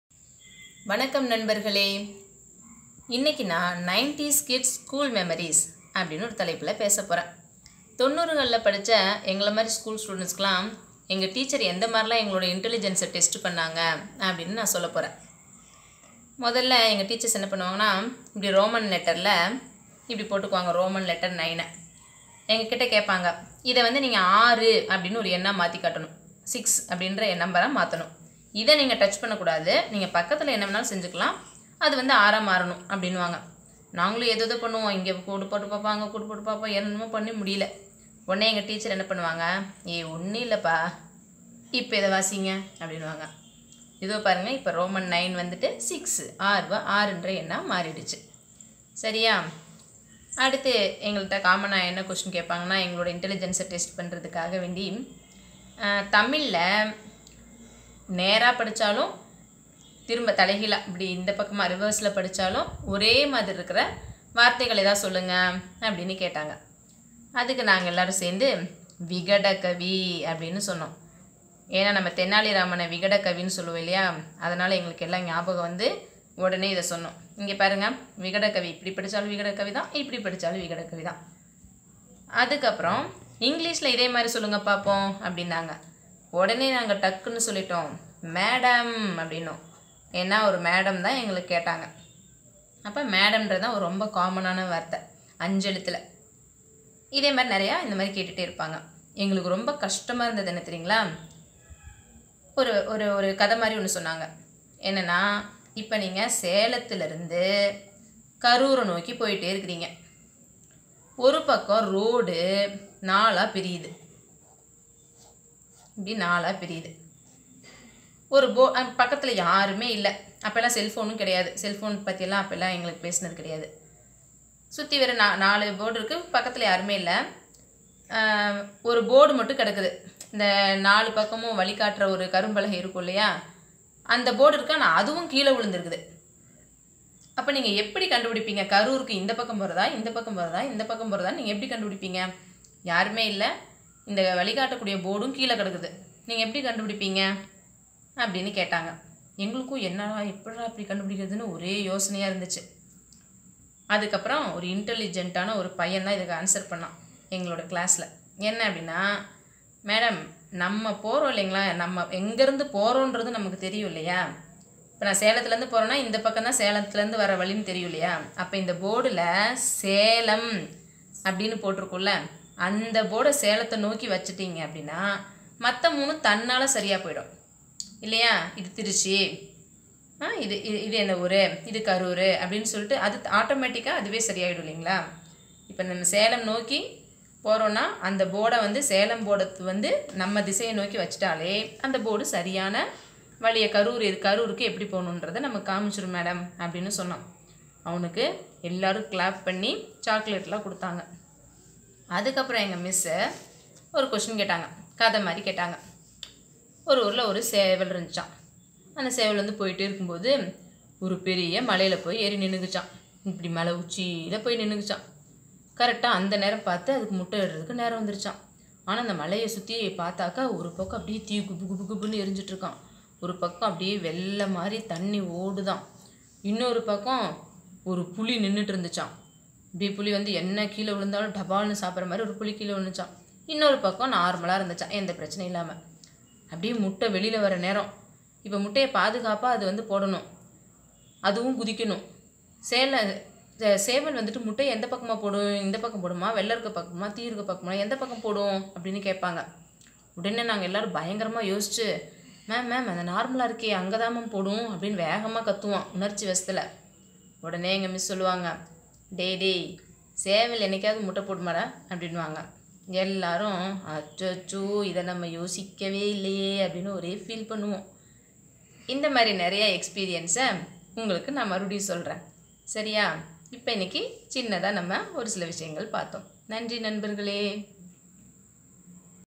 90's kids' school memories.
I described about my school experience regarding interesting questions and answer